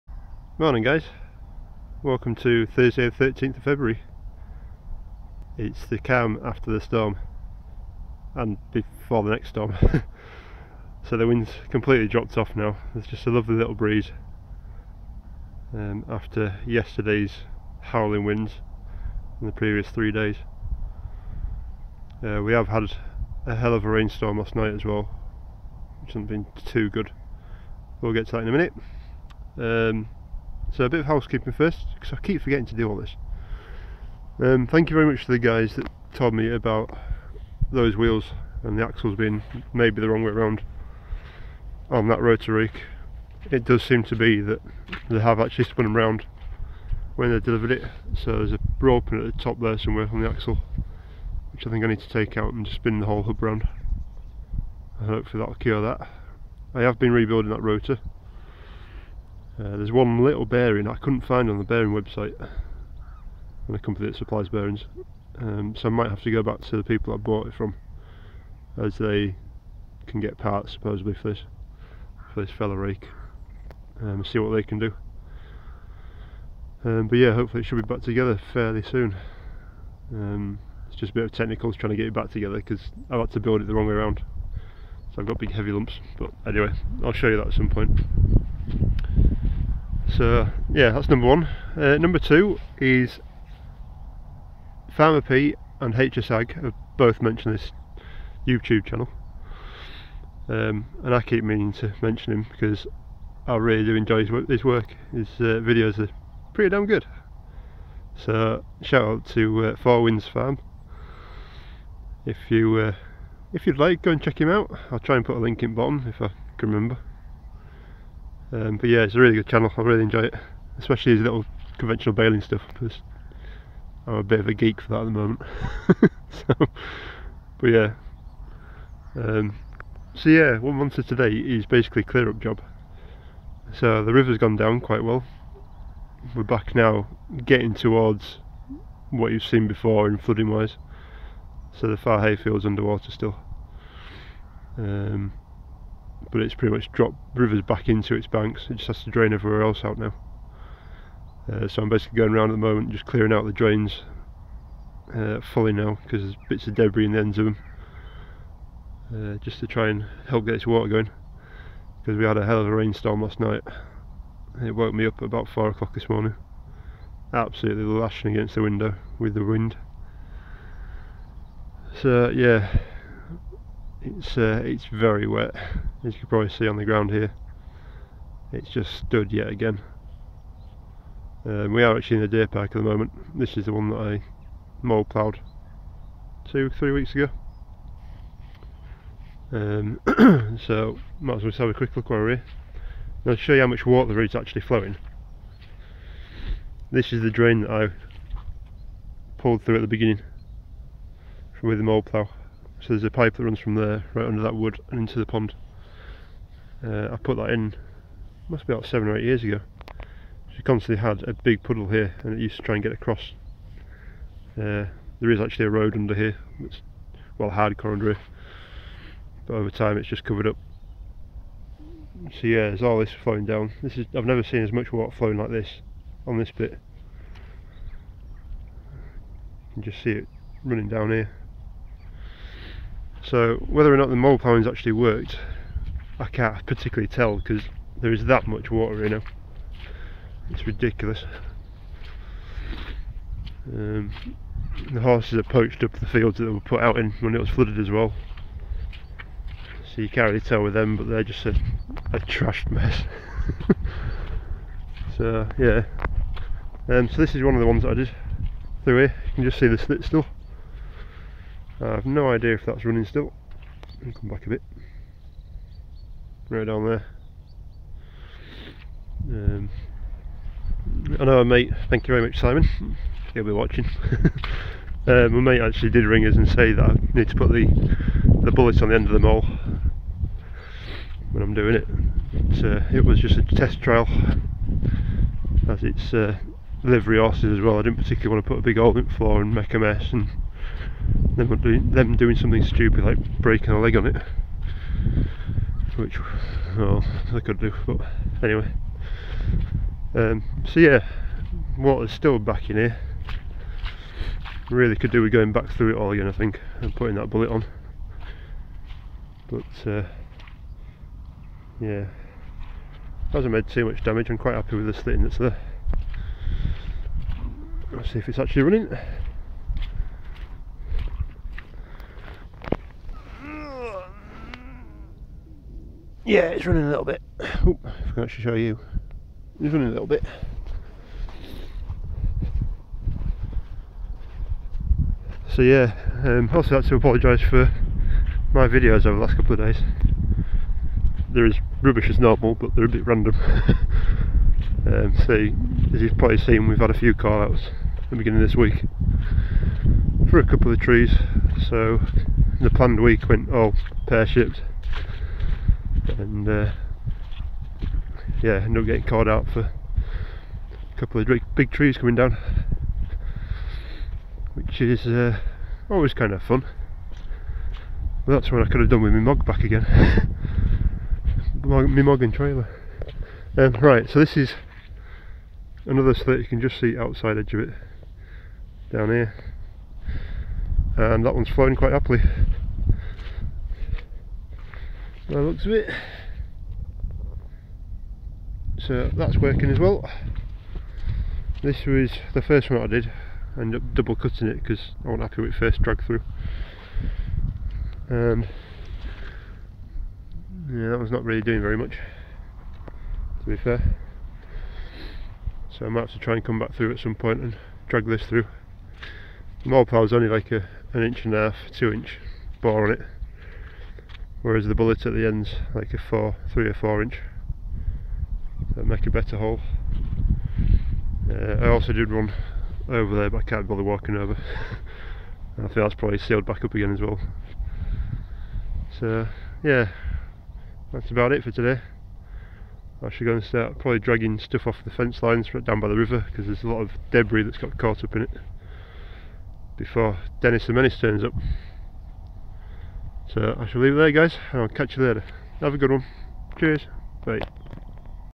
Trudging round the estate
More rain, less wind and another storm on its way! Oh the joys!